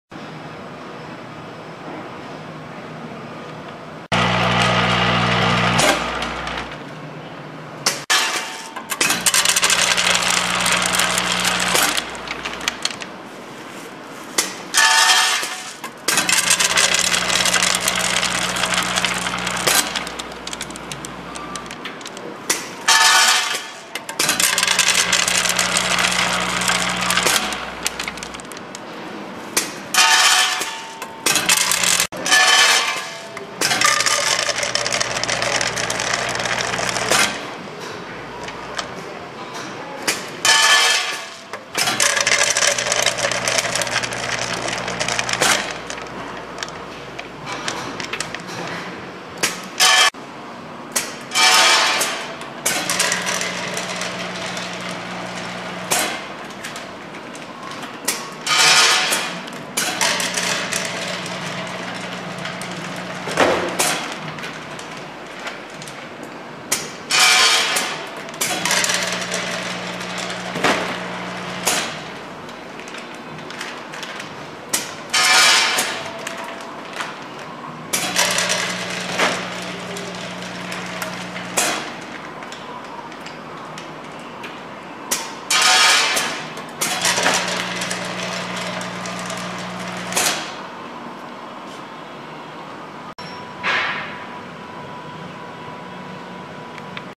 JDA- Frozen Fruit Berries Scale Filling Machine,Weigh Filler USA Canada
JDA-Scale Filling Machine,Weigh Filler , Linear Scale
Coffee,nuts, crackers, croutons, pet food, chips, buckwheat, flax seed, pastas, grated cheese etc.

JDA Progress Ind.
50 Alex ave Unit 1
Woodbridge , Ontario Canada
L4L 5X1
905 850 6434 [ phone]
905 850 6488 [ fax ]